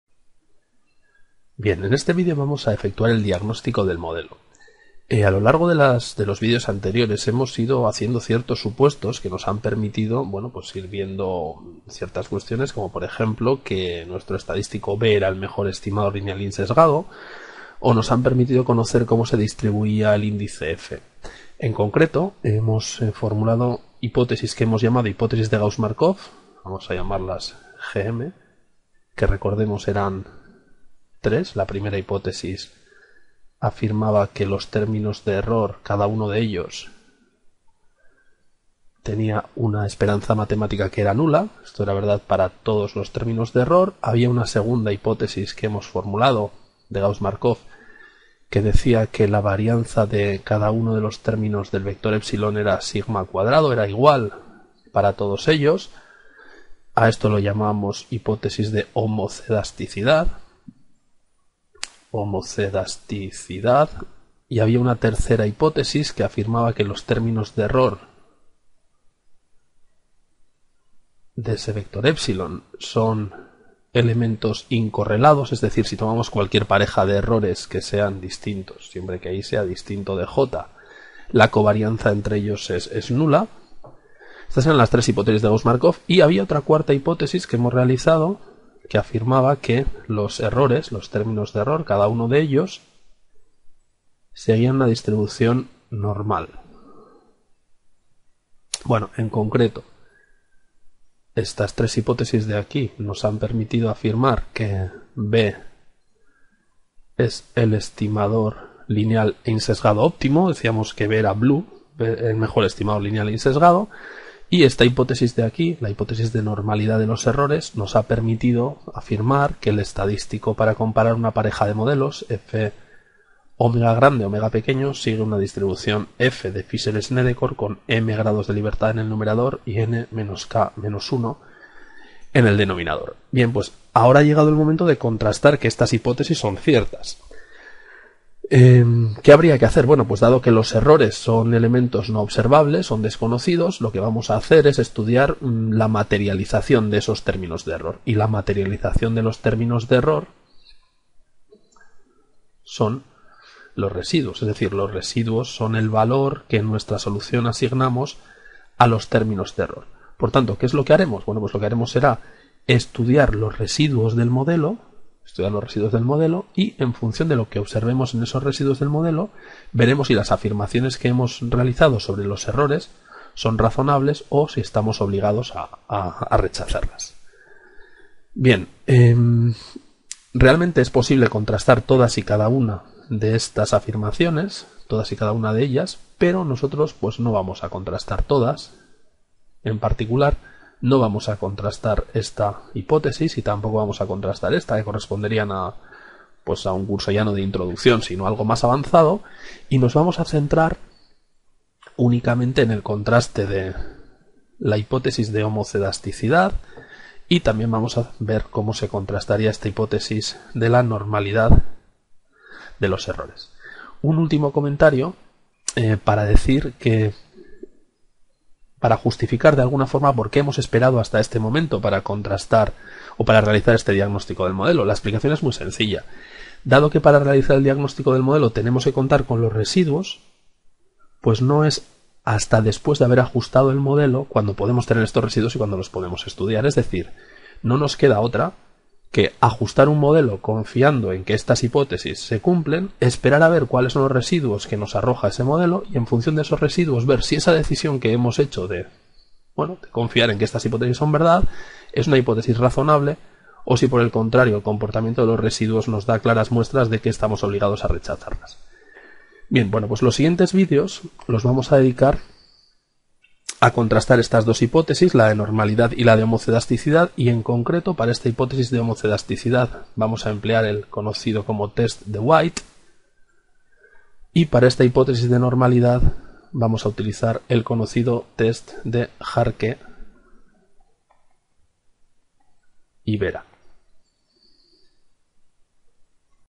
[ML-48] Diagnóstico del modelo
En este vídeo se presenta el procedimiento que se seguirá en los dos próximos para contrastar algunas de las hipótesis que hemos venido realizando sobre el comportamiento del vector de errores.

En particular, entre las hipótesis de Gauss-Markov, contrastaremos la hipótesis de homocedasticidad. Se empleará para ello el test de White.

Además, se comprobará la hipótesis de normalidad de los errores, empleando con ese fin el test de Jarque-Bera.